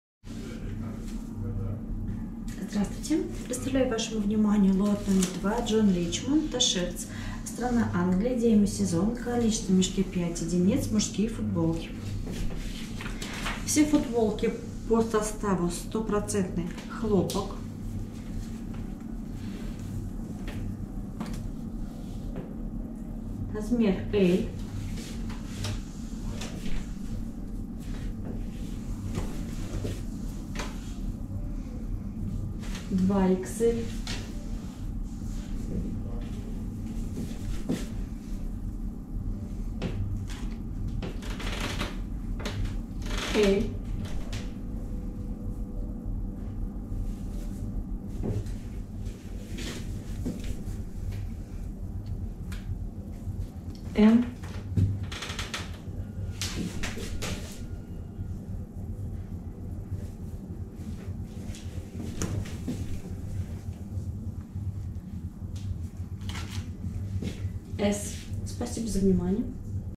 John Richmond t-shirt 2 ,Цена за ед: 14.74 евро, cток одежда оптом
John Richmond t-shirt (Лот № 2)
Футболки  мужские (cток )
Демисезон
Страна: Англия
 Цена за мешок: 73.70 евро